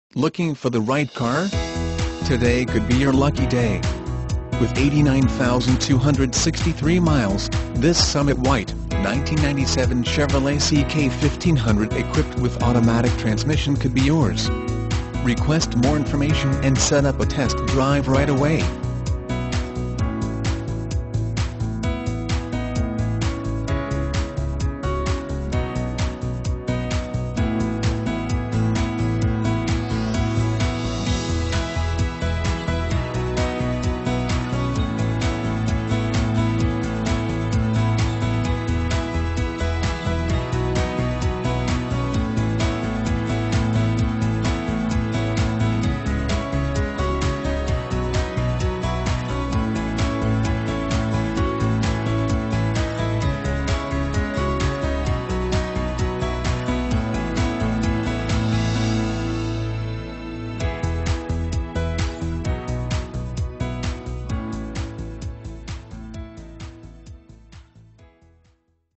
1997 CHEVROLET C/K 1500 Huntington, WV N14392A1
1997 CHEVROLET C/K 1500 Huntington, WV
Stock# N14392A1

5200 US Rt. 60 E.

Looking for the right car? Today could be your lucky day. With 89263 miles, this Summit White, 1997 Chevrolet C/K 1500 equipped with Automatic transmission could be yours. Request more information and set up a test drive right away.